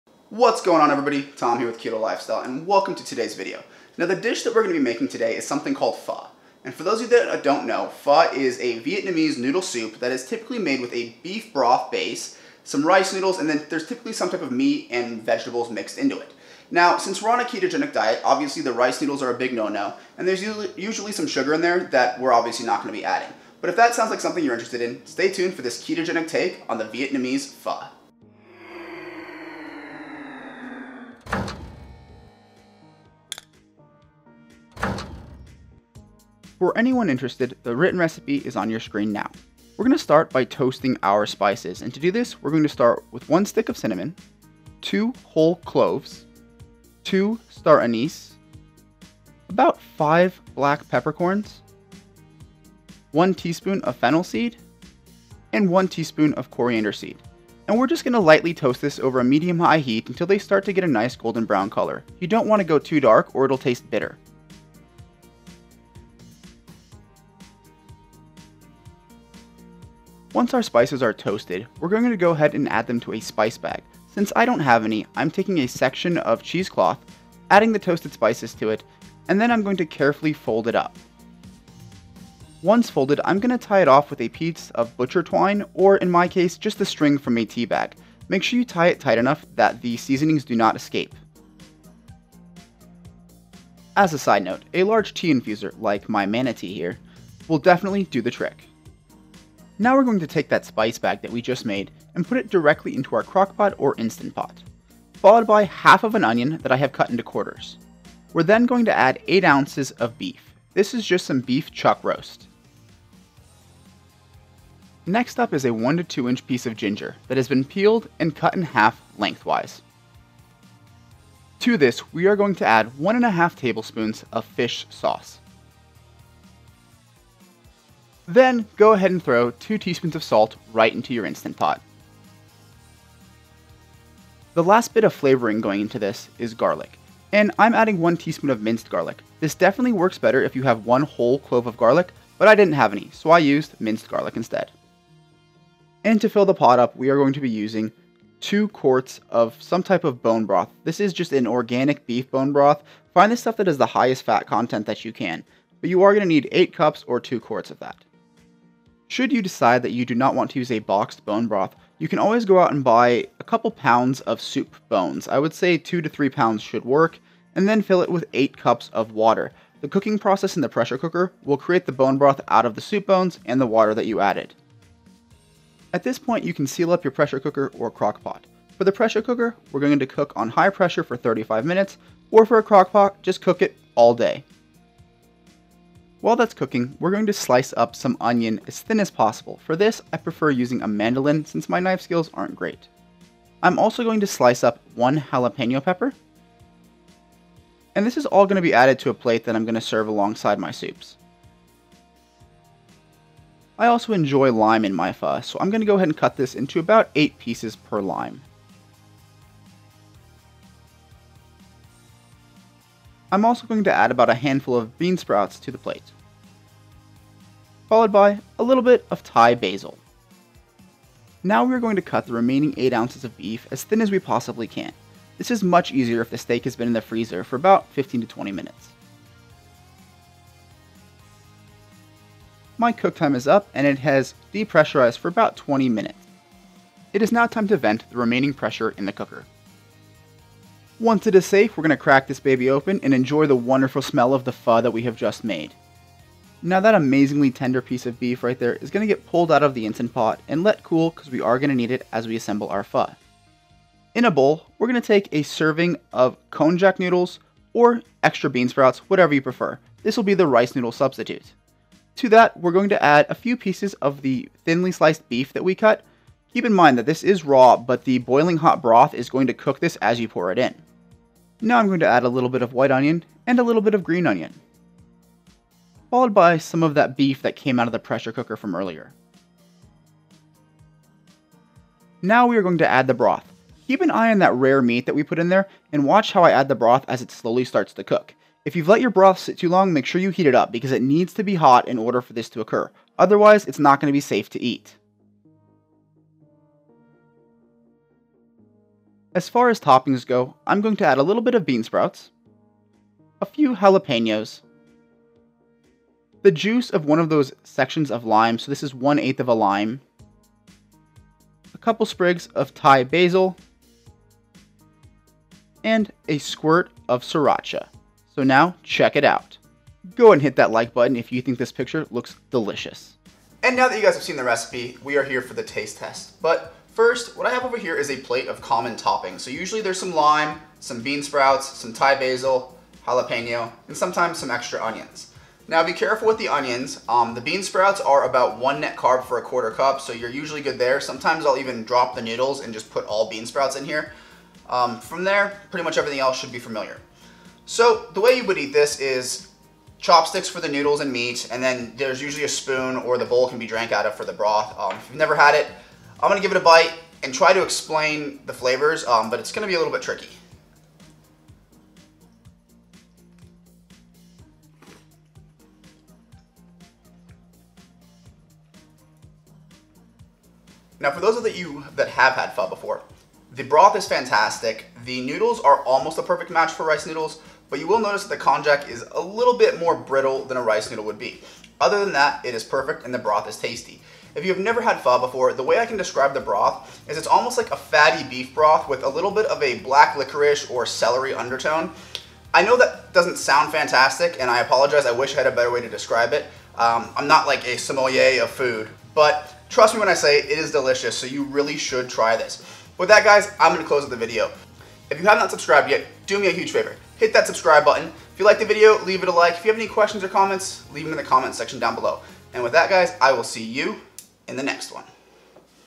How To Make Keto Pho | Keto Pho Recipe | Keto Noodle Soup Recipe
Pho has always been one of my late night favorites. Though many places offer it at all hours of the night, I have never found a keto compliant version. The rice noodles and sugars simply give it too many carbs. If you find yourself craving pho, this keto pho recipe will surely hit the spot.

Servings: 4
Macros per serving:
Calories: 443
Fat: 30g
Total Carbs: 7g
Net Carbs: 3g
Protein: 39g

Note: Bone broths and beef cuts vary a lot on fat, protein, and carb content. Be aware of what you're using. Macros are calculated assuming no toppings and using the macros listed on the ingredients that I used.